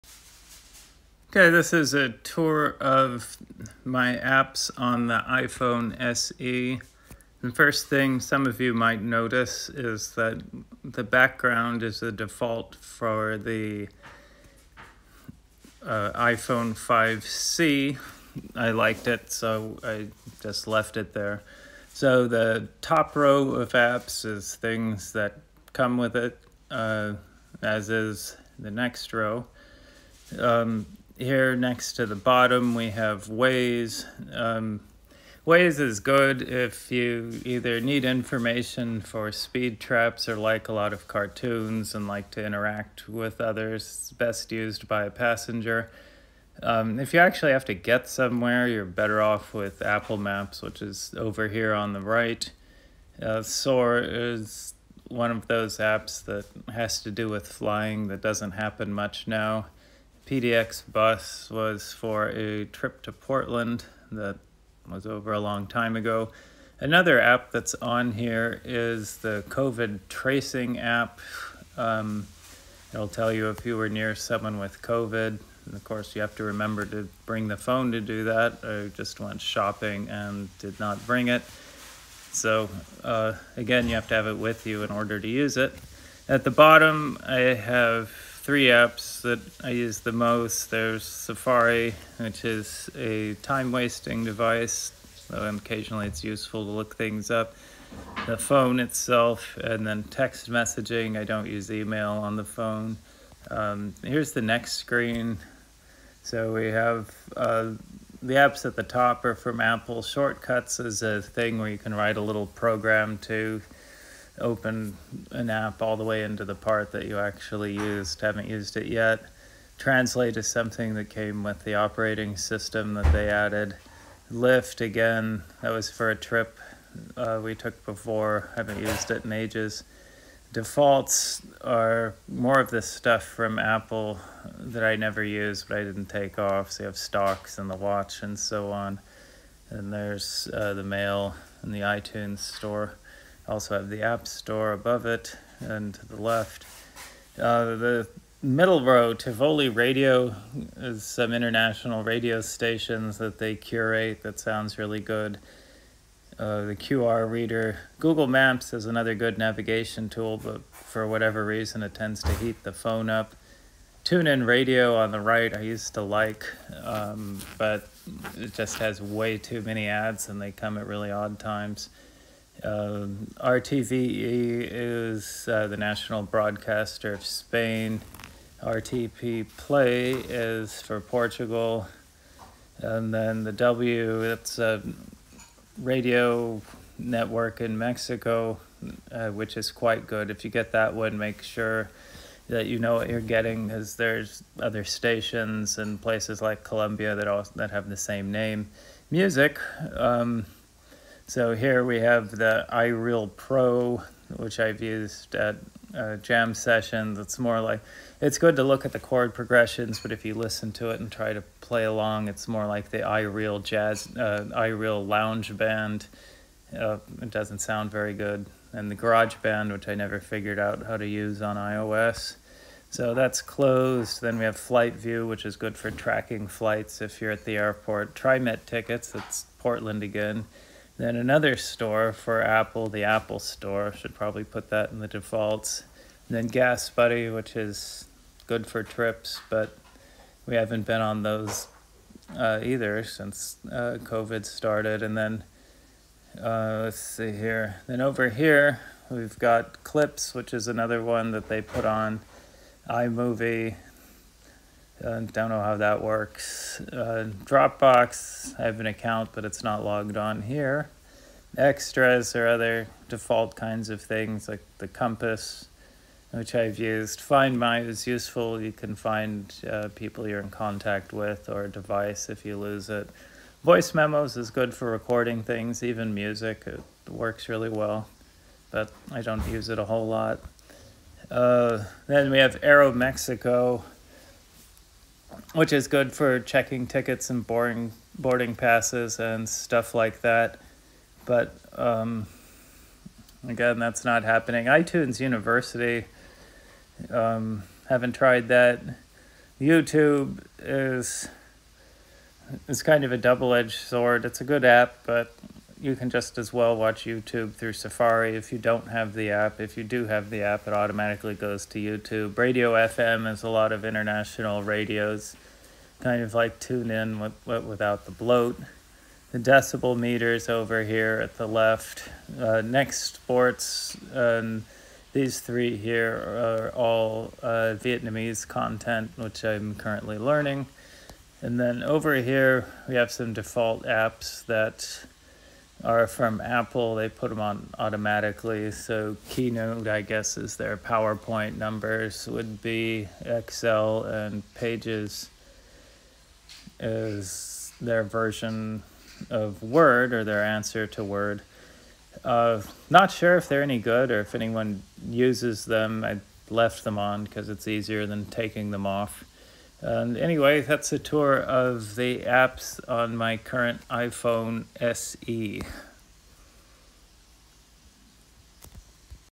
A tour of my apps on the iPhone SE
There is one app shown that I never mentioned: screen recording. If you use it, hold down the recording button first to make sure that the microphone is on. Apple’s default is to leave it off.

This tour of the apps will help anyone who watches my reviews. You can see how your own use cases line up with mine, or not, and then make your buying decisions.

Apps mentioned by name: Waze, Apple Maps, Soar, PDX Bus, COVID tracing, Safari, Shortcuts, Translate, Lyft, Stocks, Watch, Mail, iTunes Store, App Store, Tivoli Radio, QR Reader, Google Maps, Tune-In, RTVE, RPT Play, W Radio (Mexico), iReal Pro, GarageBand, FlightView, TriMet Tickets, Apple Store, Gas Buddy, Clips, iMovie, DropBox, Compass, Find My, Voice Memos, AeroMexico, iTunes U, YouTube, Radio FM, dBMeter, NextSports, Keynote, Numbers, Pages